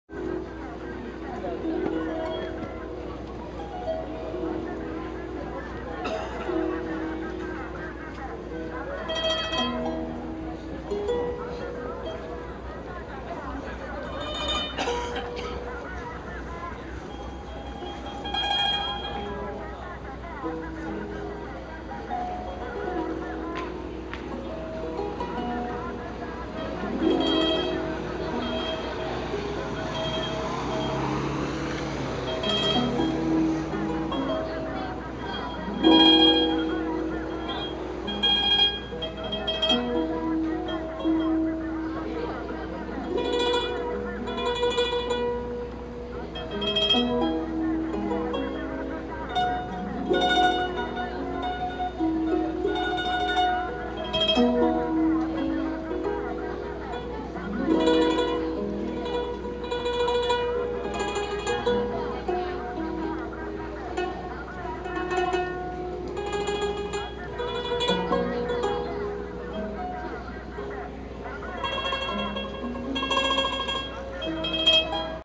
Street music live in Taipei
This lady was playing beautifully along Tamsui river.